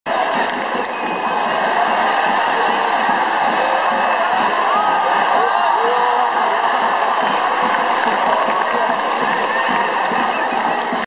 2011 J2 第38節 ファジアーノ岡山 前半 妹尾選手のクロス
徳島ヴォルティスとの最終戦、妹尾選手のクロスから、石原選手へ。